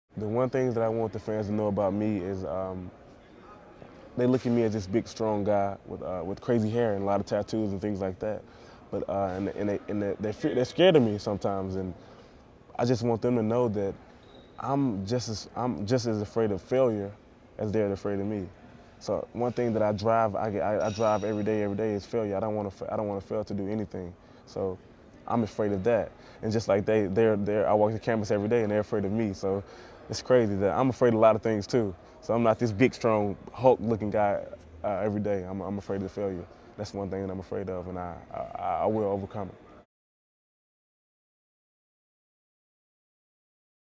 Jae Crowder Talks About Biggest Fear
Junior Jae Crowder talks about his biggest fear, failing.